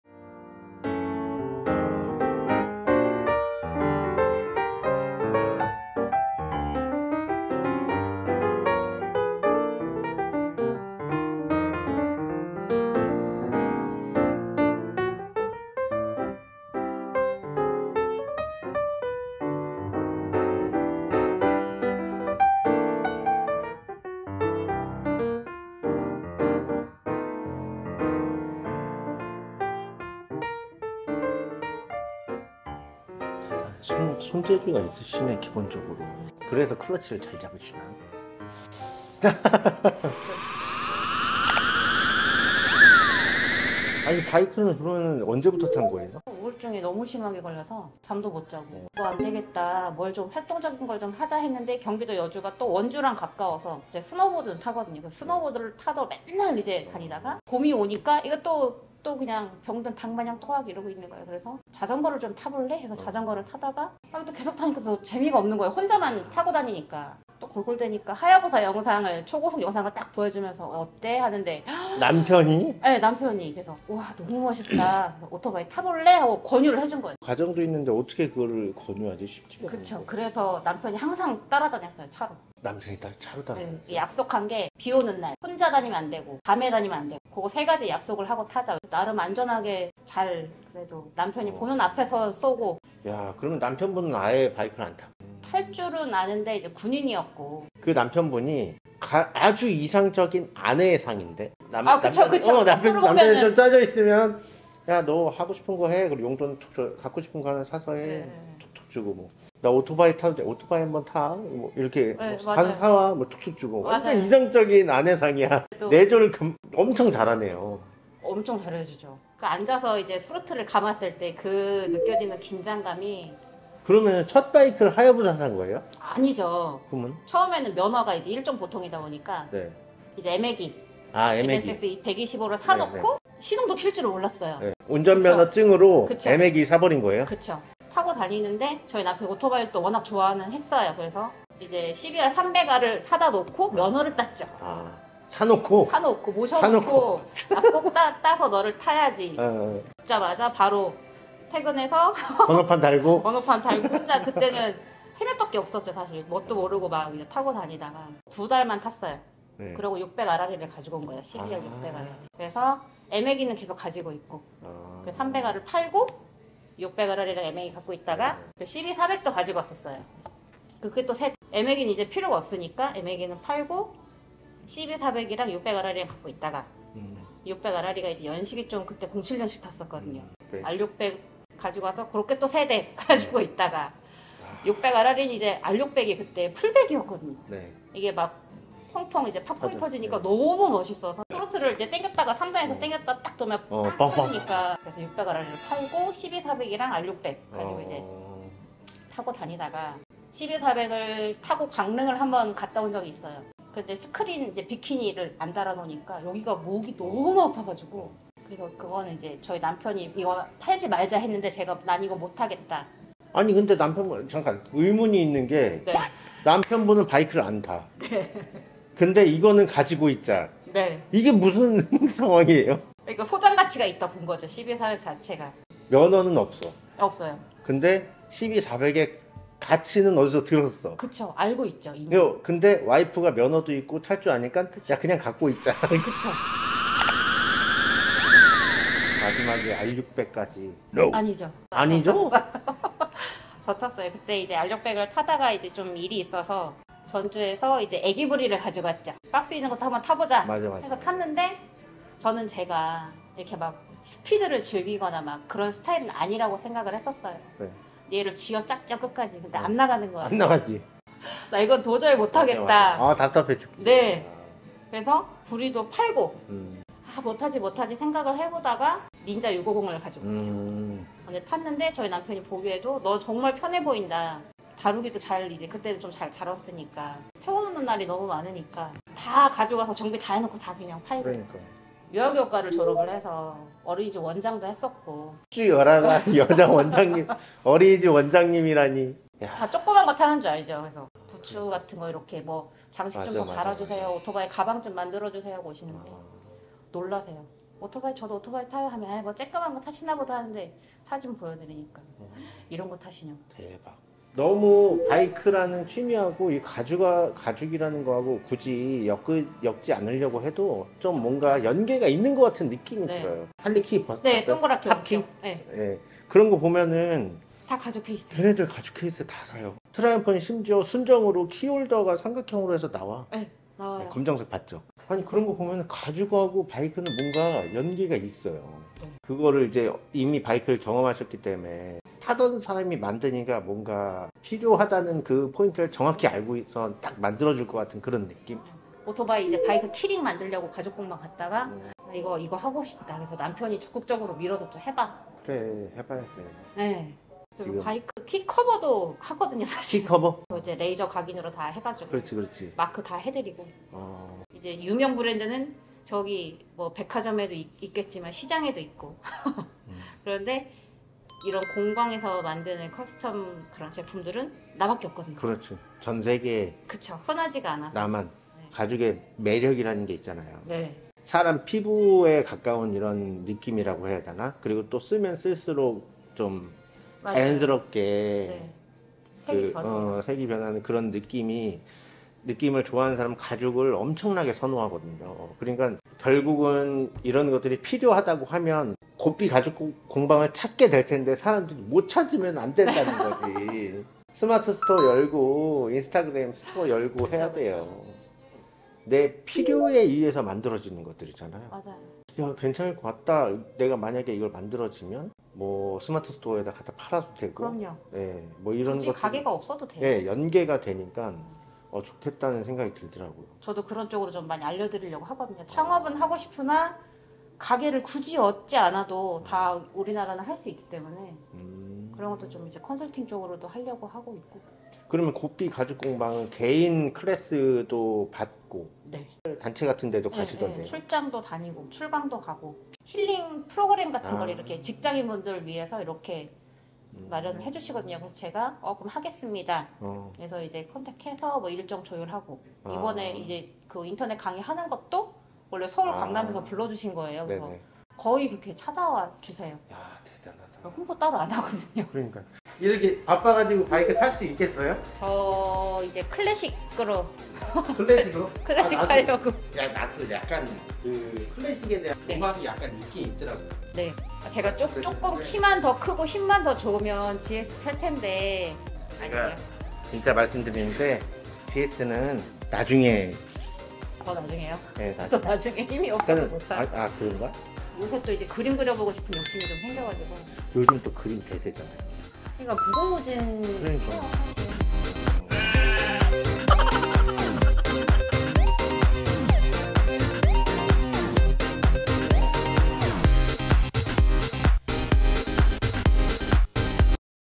어린이집 원장님이라고 바이크 타지 말라는 법 있나요? 폭주하듯 달린 바이크스토리 그리고 이번에는 또 ??
바이크를 입문하고 즐기고 이제 라이더를 위한 맞춤 가죽공방까지